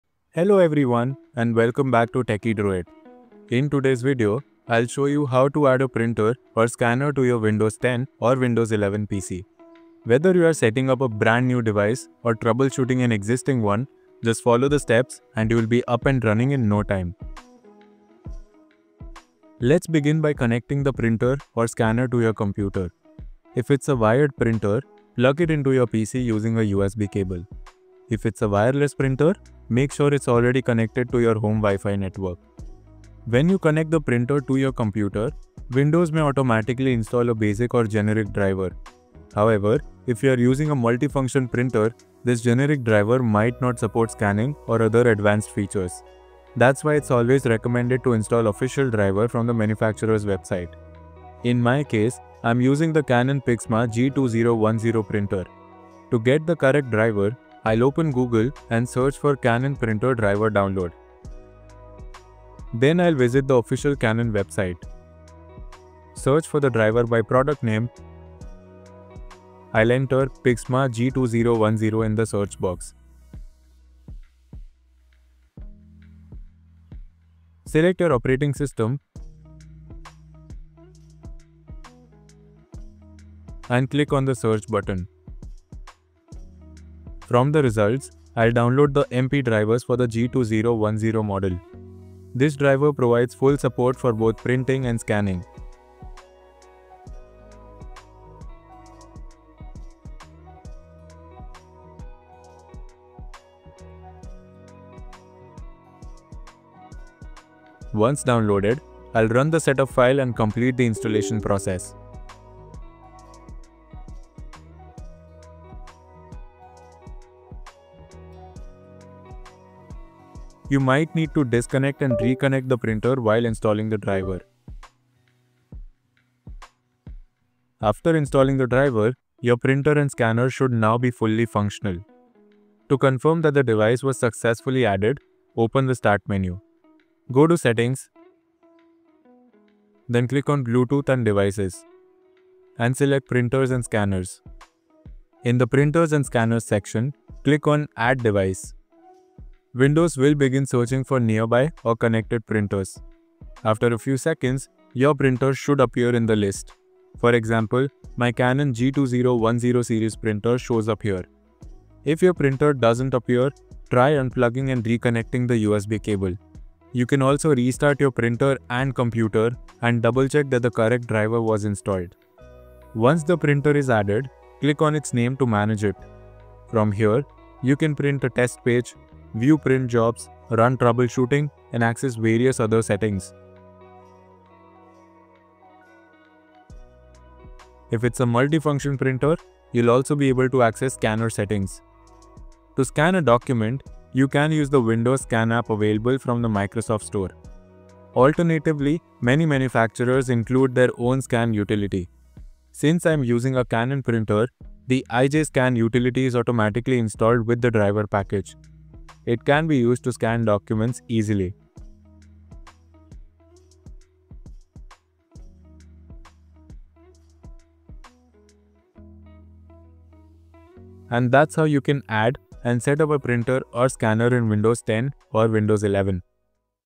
How to Add a Printer or Scanner in Windows PC
In this video, I’ll guide you step-by-step on how to add a printer or scanner to your Windows 10 or Windows 11 PC — whether you're setting up a new printer, fixing an existing one, or installing the correct driver for full functionality.

💡 Quick Tip:
 Windows often installs a basic driver automatically, but if you're using a multifunction printer, install the official driver to unlock scanning and other features.

🖨️ Example Printer Used: Canon Pixma G2010
 ✅ Print & Scan tested using Windows Settings and IJ Scan Utility